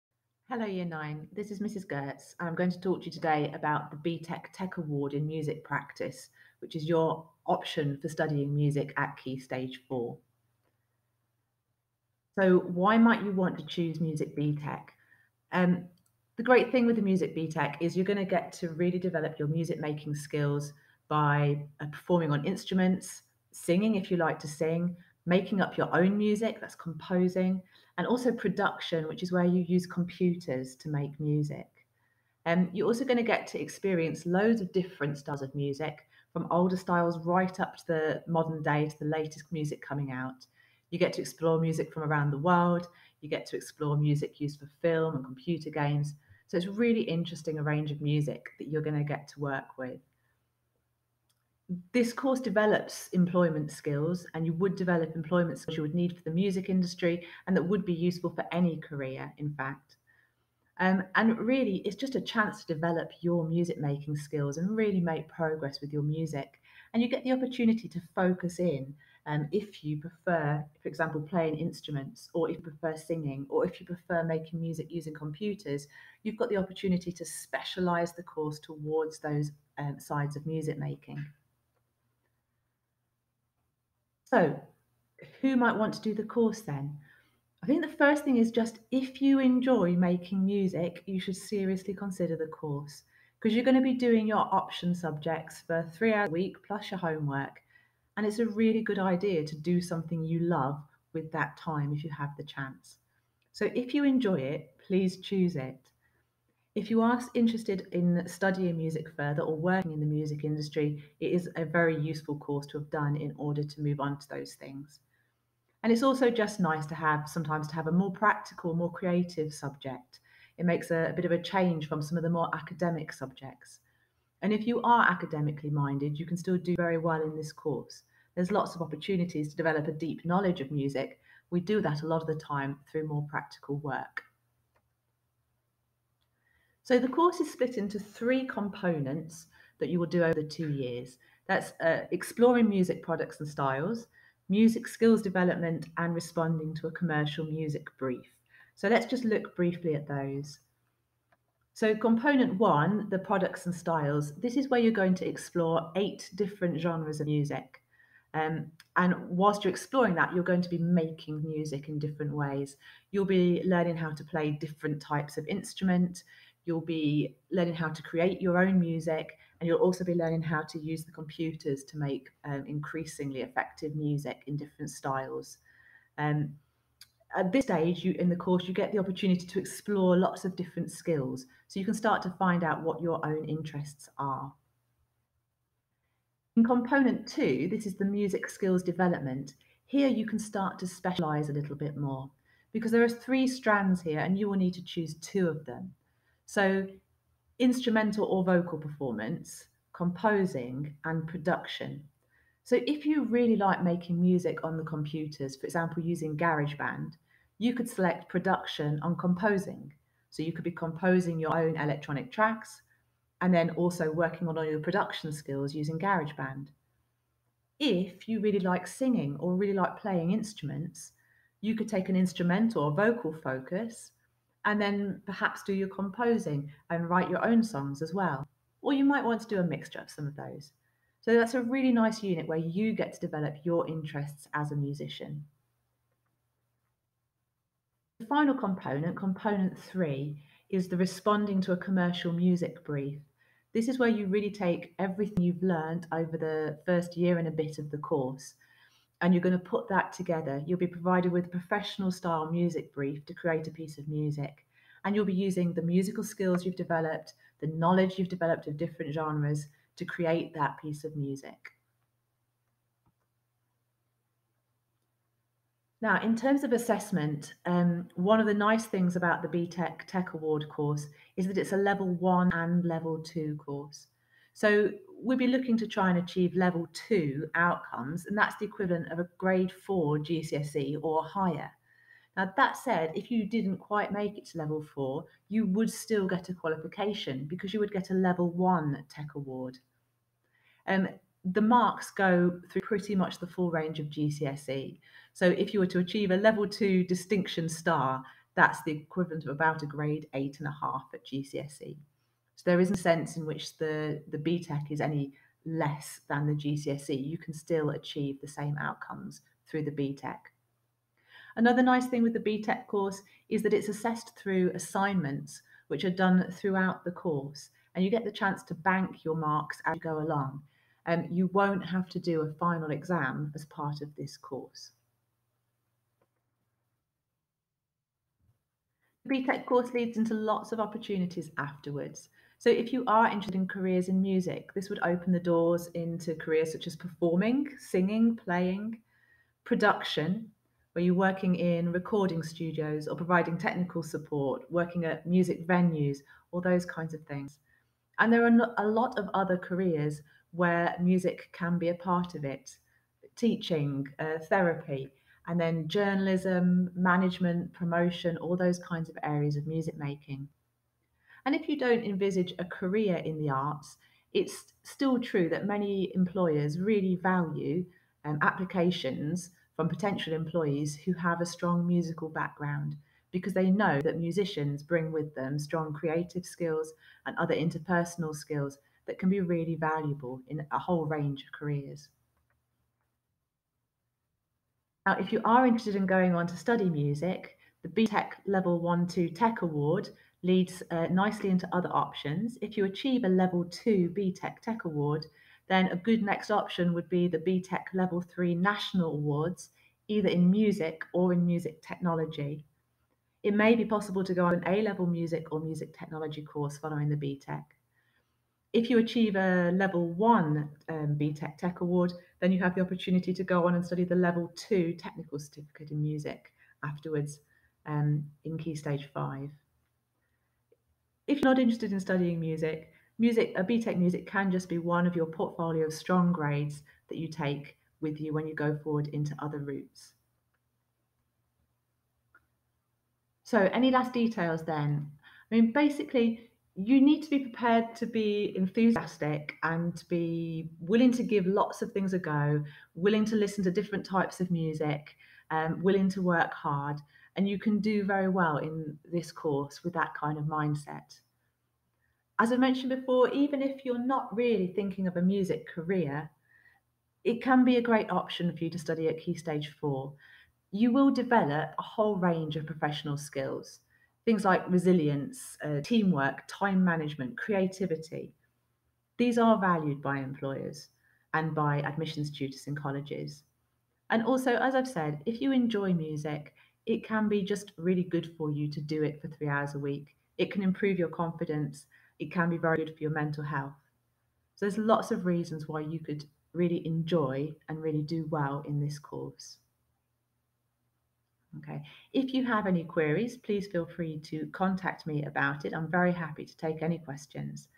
YR 9 Options - BTEC Music
This video outlines the BTEC course for Music offered at HLA